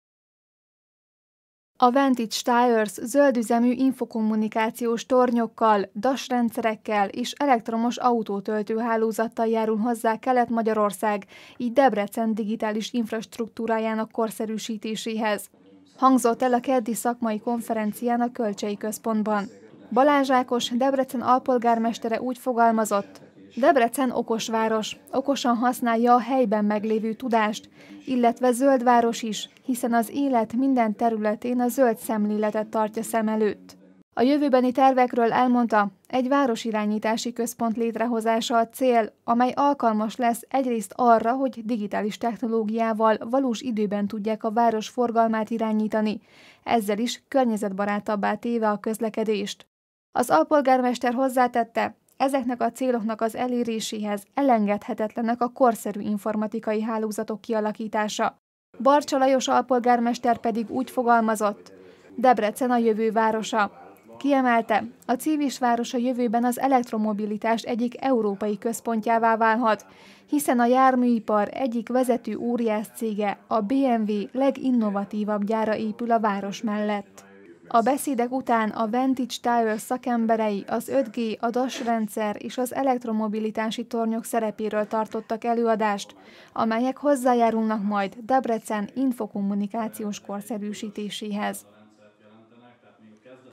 Fókuszban a digitális jövő - Debrecenben fejleszt a Vantage Towers
A Vantage Towers zöldüzemű infokommunikációs tornyokkal, DAS rendszerekkel és elektromos autótöltő hálózattal járul hozzá Kelet-Magyarország, így Debrecen digitális infrastruktúrájának korszerűsítéséhez – hangozott el a keddi szakmai konferencián a Kölcsey Központban.